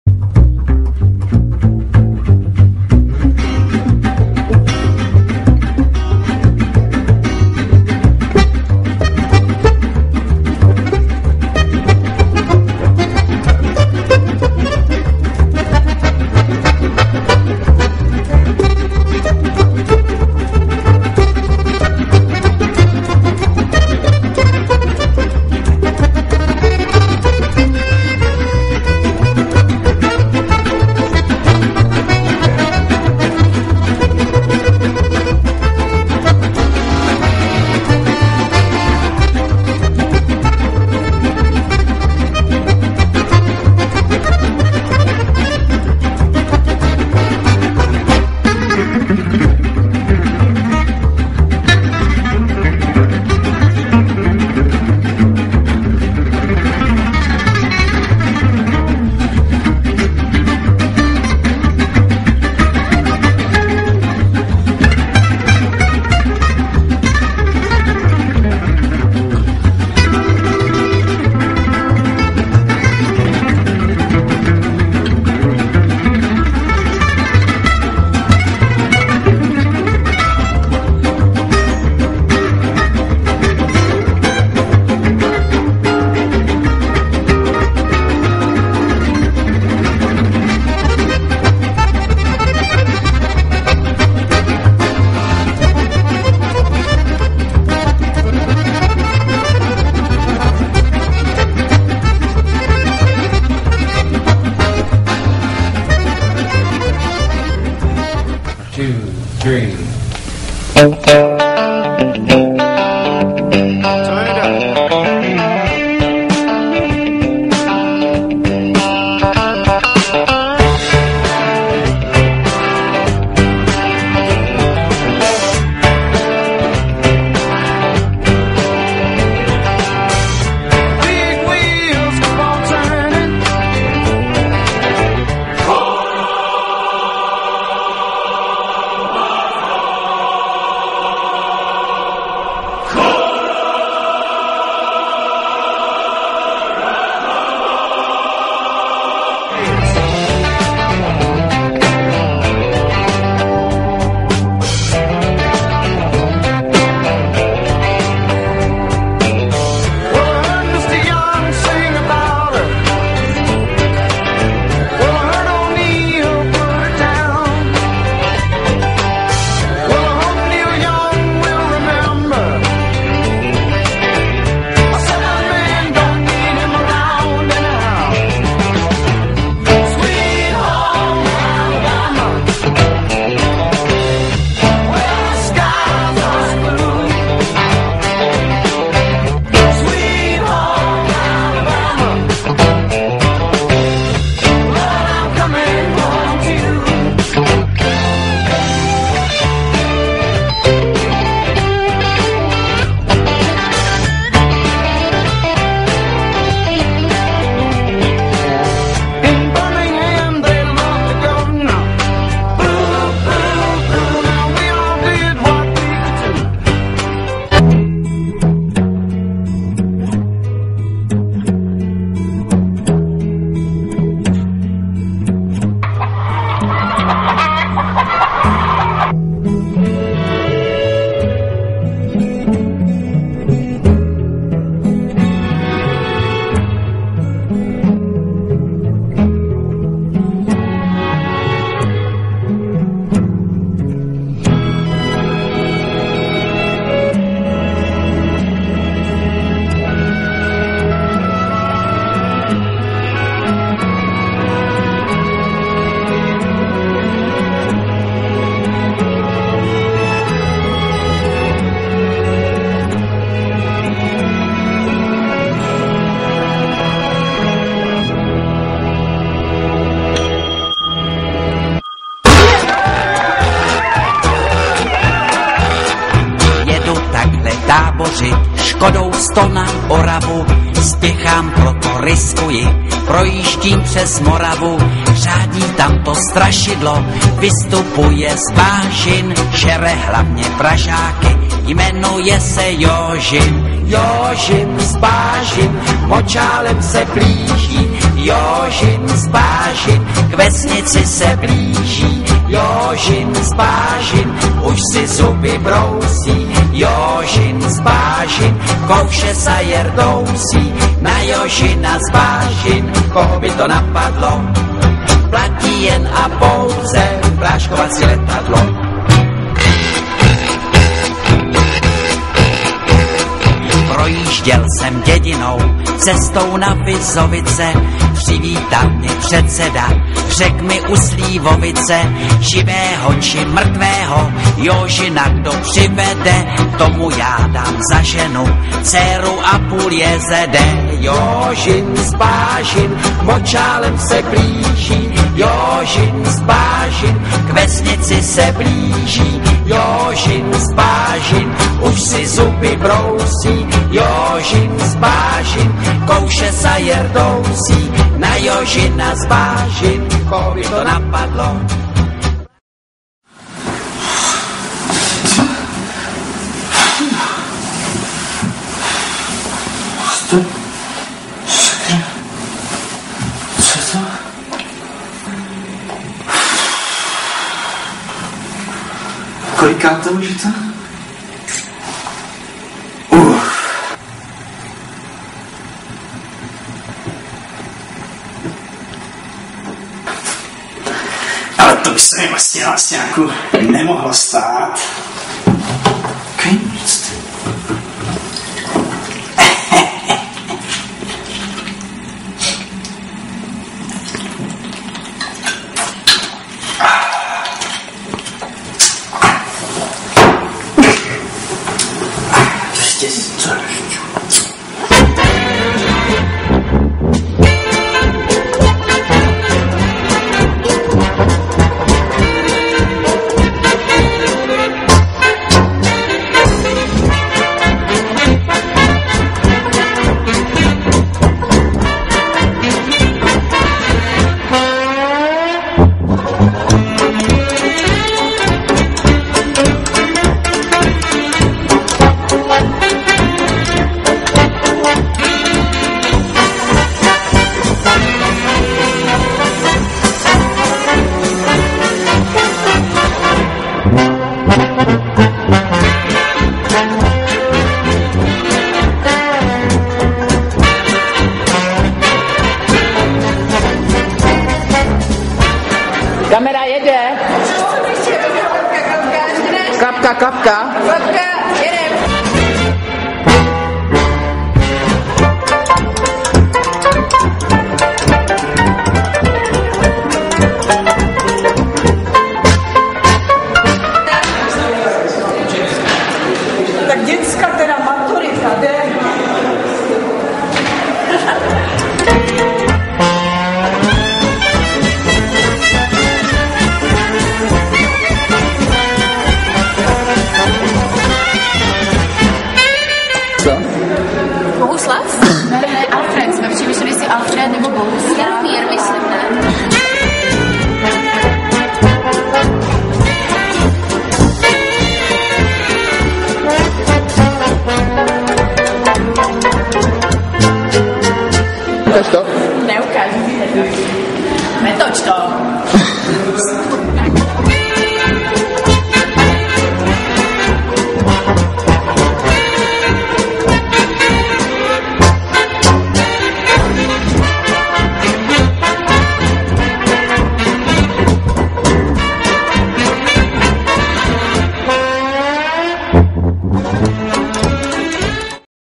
Co ňa čeká, to ňa nemine - Část 2 - Živé jazyky Gymzl 2010
Veleúspěšný velkofilm z dílny Markéty Tiché a Jana Kotíka. Film je tak trochu o nás všech, proto příjemnou podívanou!